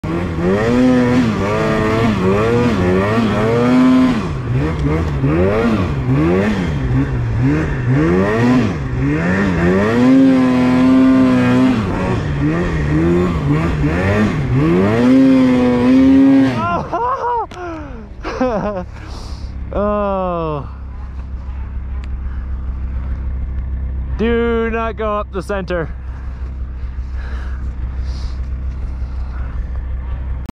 Z BEAM STUCK
Zak Collings sticks his sled's 'BBP' (Basic Balanced Position) - demonstrating positivity in every riding situation.